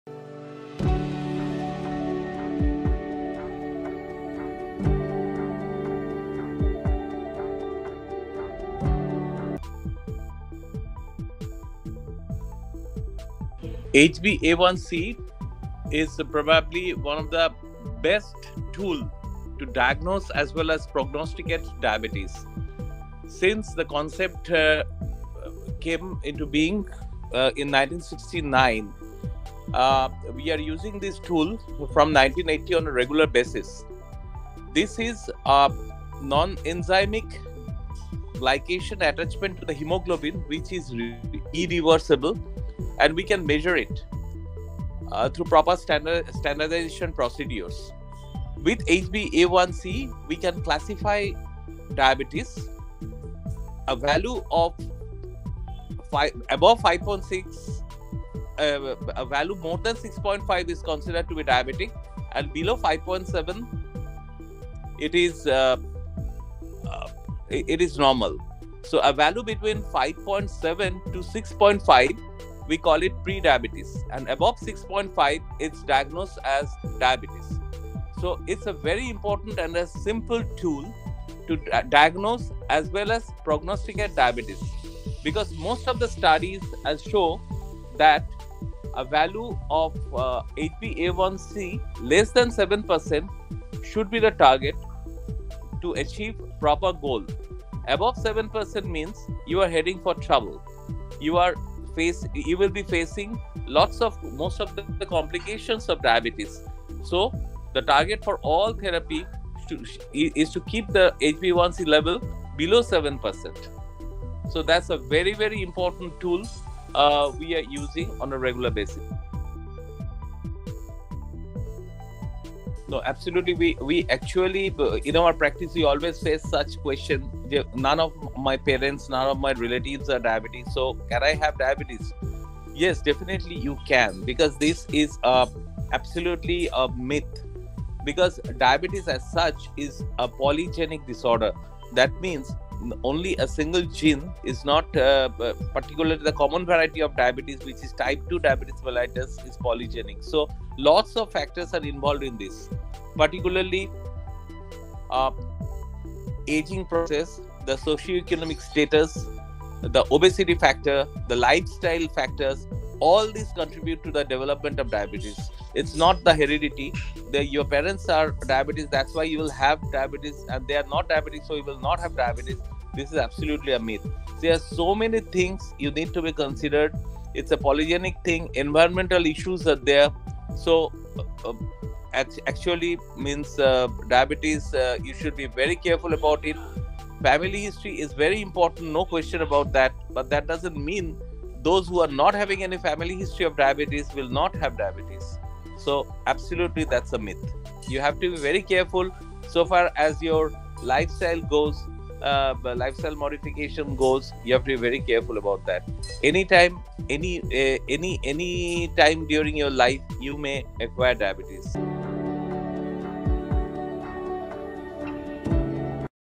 Is Diabetes-Free Family History a Guarantee Against Developing the Disease? -Dr Arijit Roy Chowdhury
The HbA1c test measures average blood sugar levels over several months, offering a comprehensive view of diabetes control. This test is crucial for assessing the effectiveness of management strategies. Maintaining HbA1c below 7% is essential, indicating well-regulated blood sugar levels and reducing the risk of complications such as heart disease and kidney issues. Regular testing aids in informed treatment decisions and prevention of long-term complications.

Contrary to the popular myth that not having diabetes in the family guarantees immunity, it's essential to recognize that diabetes can still develop. While family history is a risk factor, lifestyle and environmental factors also play significant roles. A sedentary lifestyle, poor dietary choices, and other health factors contribute to diabetes. Therefore, regardless of family history, maintaining a healthy lifestyle and regular health check-ups are crucial for preventing diabetes and promoting overall well-being.

In this video, Dr. Arijit Roy Chowdhary, Consultant Physician and Cardiologist at Helen Cross Diagnostics, Kolkata, discusses about the HbA1c test and it’s importance in treatment of Diabetes and confronting the misconception that one is not prone to Diabetes if no one in the family has it.